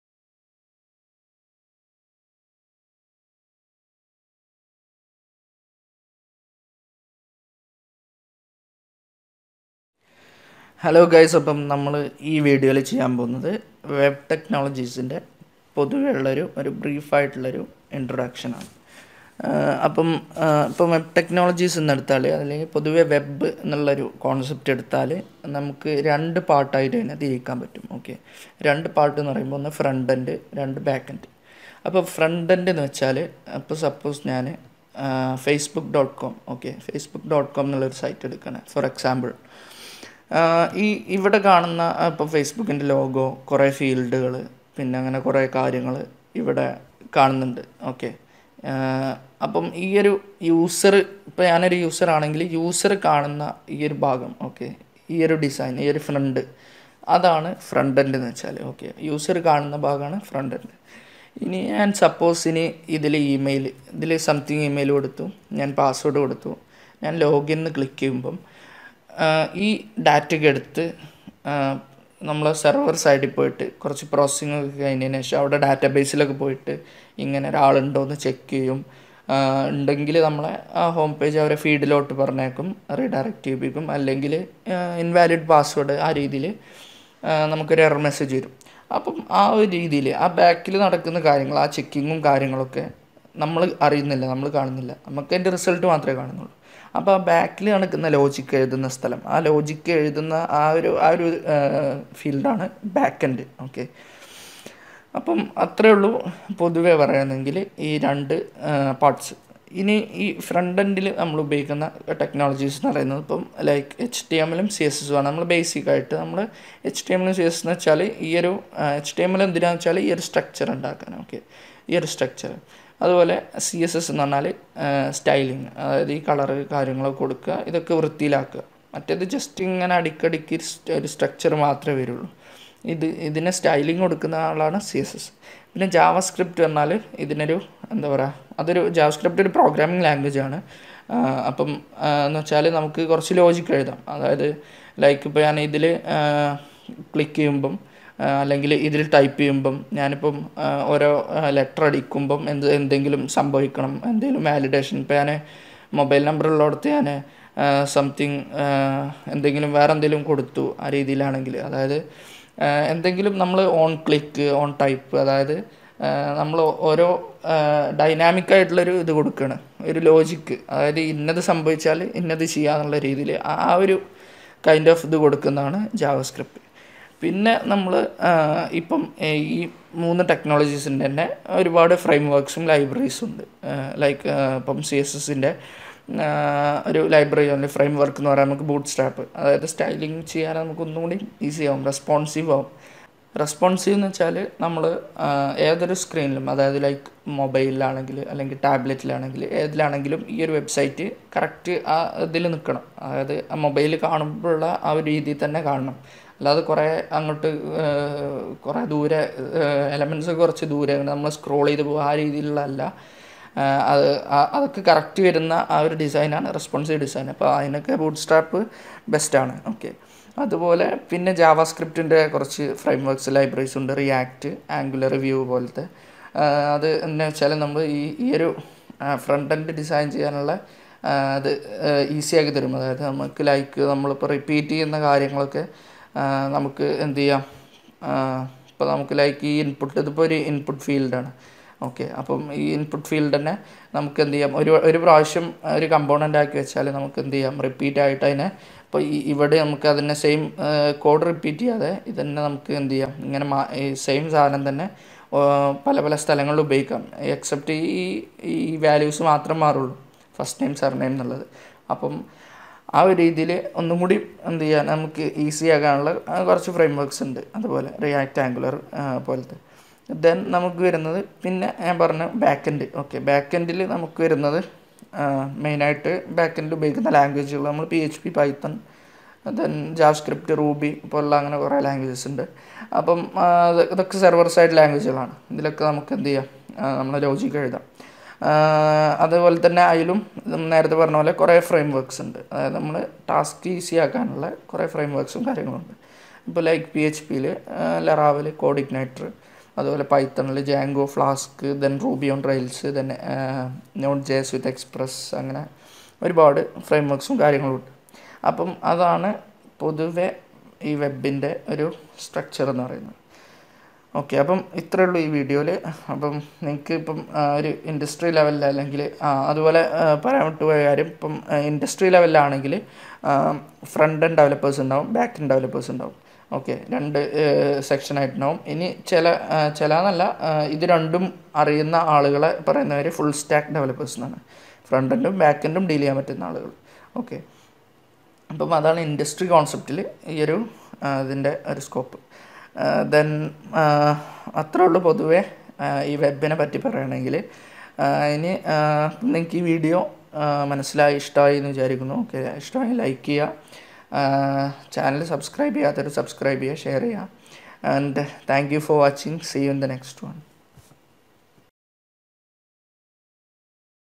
Malayalam | Front-End, Back-End & Full Stack Web Development
In this video, I will walk through the learning path of becoming a complete web developer. We'll talk about front-end & back-end with the technologies used in both. We will also discuss about Full Stack Development and it's role in industry.

Stay with us and enjoy!